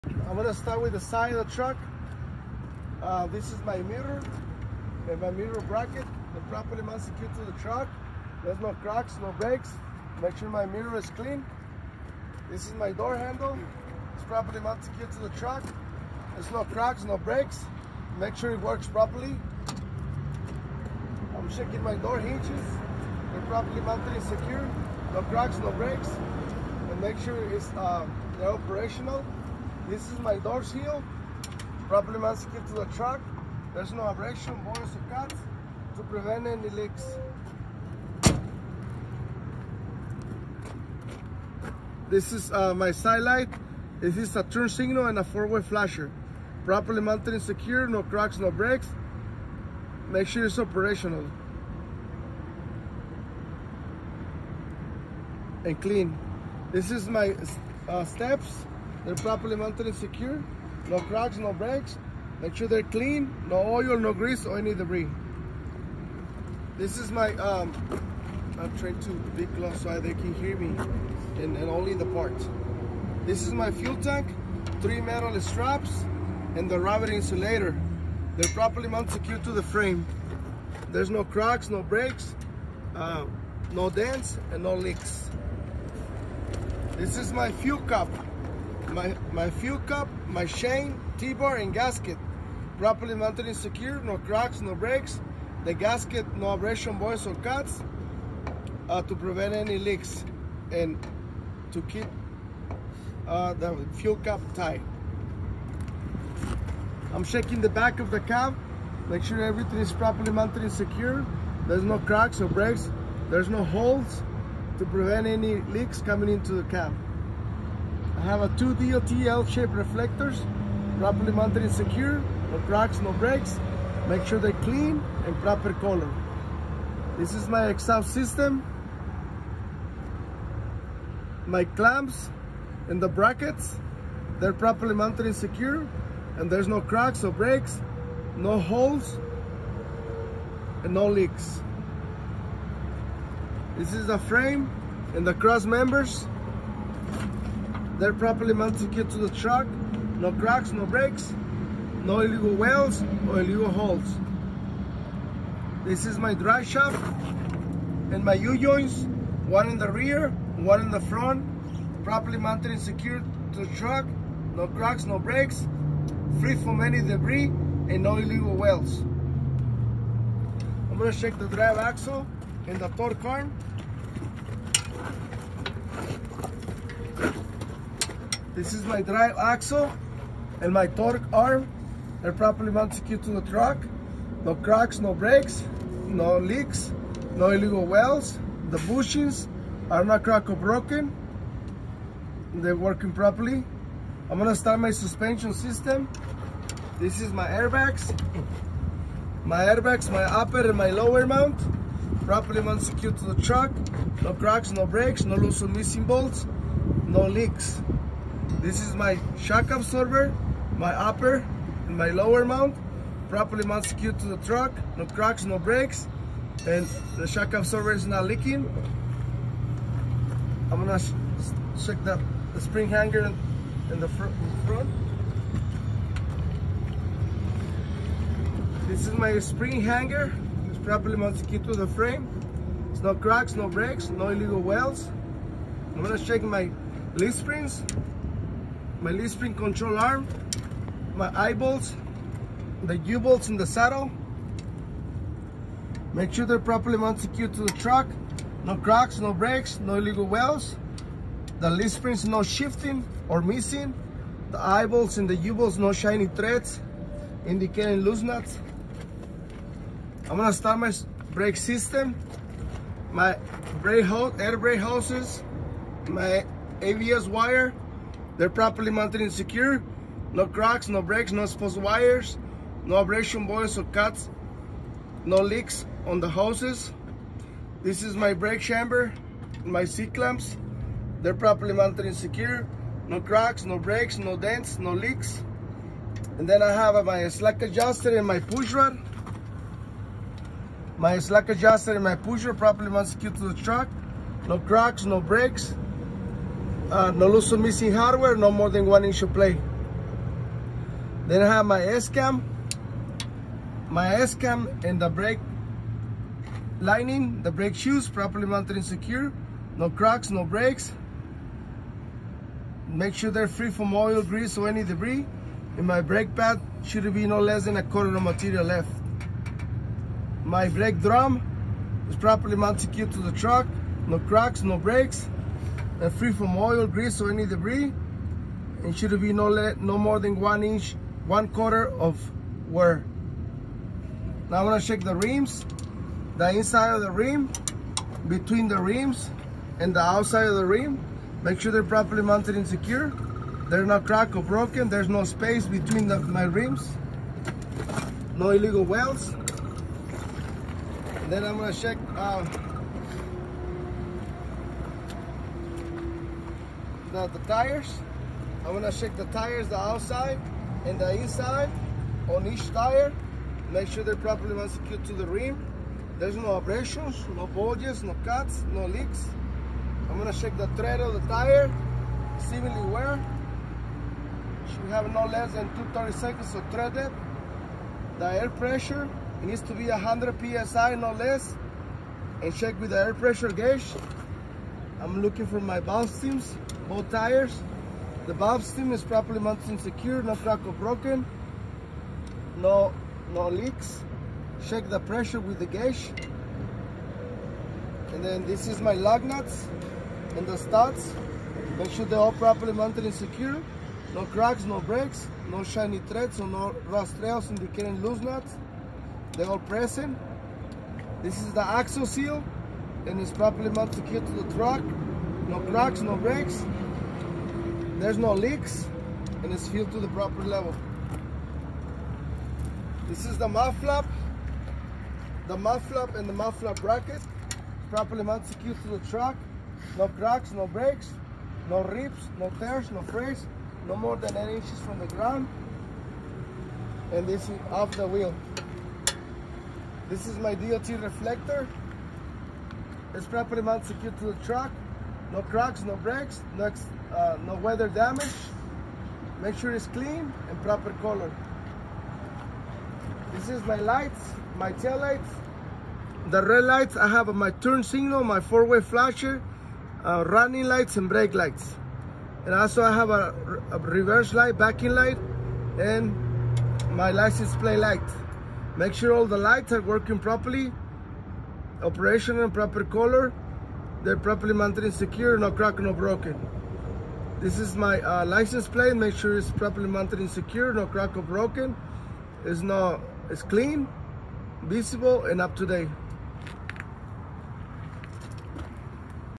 Pre trip Inspection Manual-tractor driver door to the rear of the truck
CDL INSTRUCTION